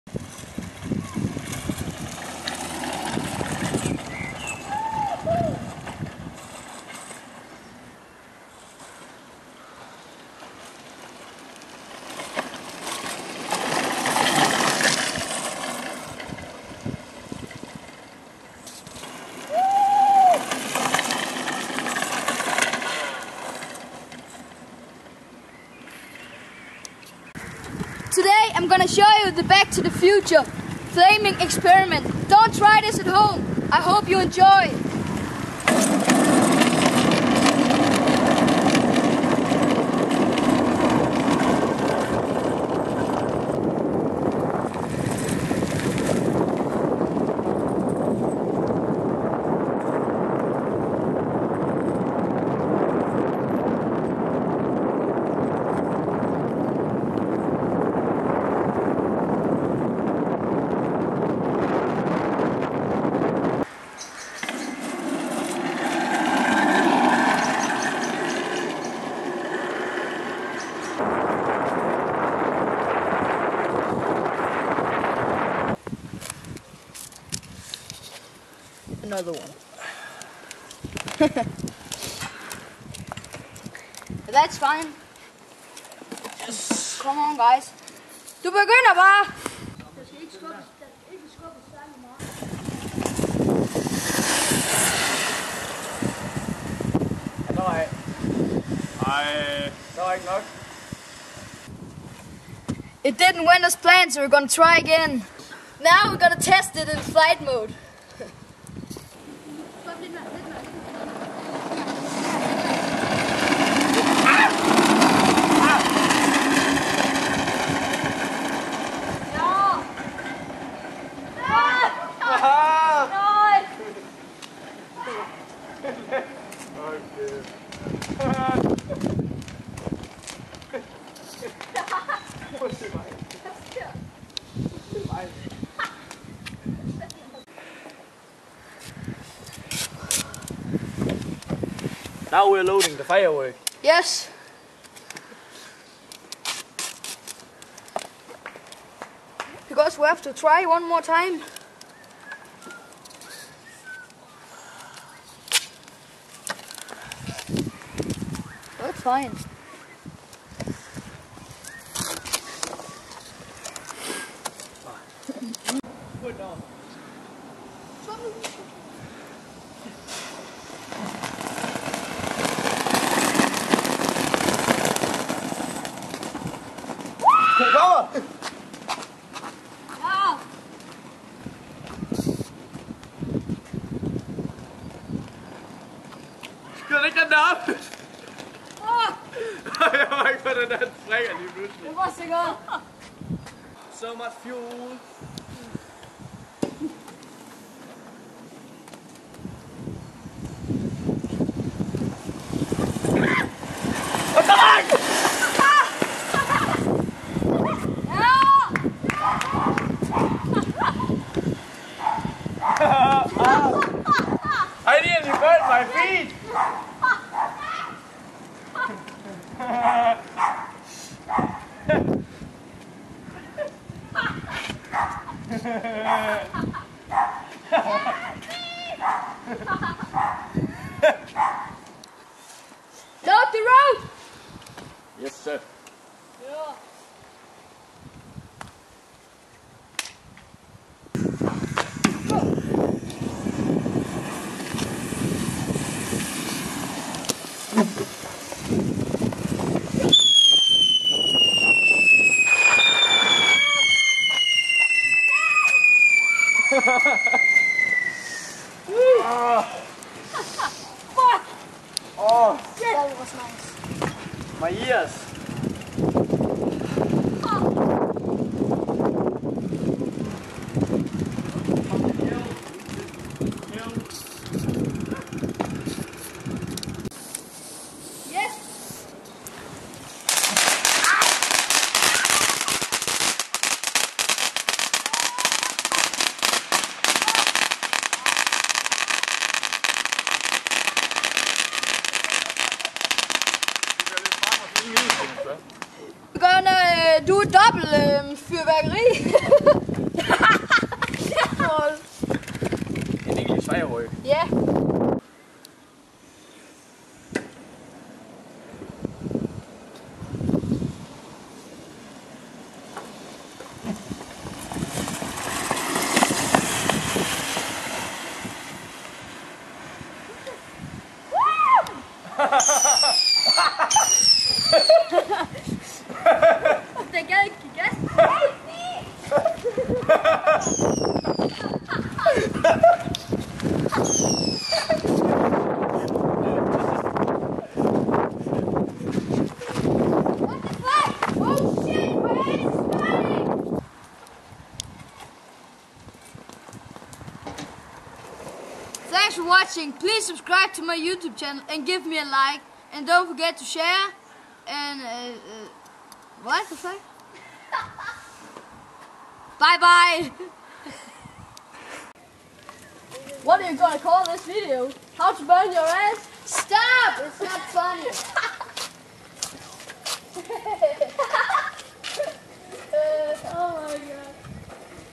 Back To The Future Go Kart Flaming Experiment
Today I'm going to show you how to make flames on the asphalt like in the movie back to the future when the DeLorean Car was traveling in time. I have invited a few friends to help me with the experiment. We are using fireworks, safety glasses and some slow burning liquid put on the asphalt. In the first part of this video I'm also going to show you a demonstration what this homemade back to the future go kart can do. With almost 40 km an hour I'm going to show you with the double steering system how to change from normal steering to flight mode and with the normal steering the speed limit reach almost 50. I hope you enjoy this action-packed YouTube video 20,000 subscribers special as we are going to take it to the maximum.

Special thanks to all my subscribers I really appreciated and thank you for all your comments and likes on my video.

And a special thanks to my friends for helping me out with this video:

Emil Marchholm.
Emil Guldbæk.
Rasmus Møller.

Copyright : Kim Henriksen And Benjamin Henriksen.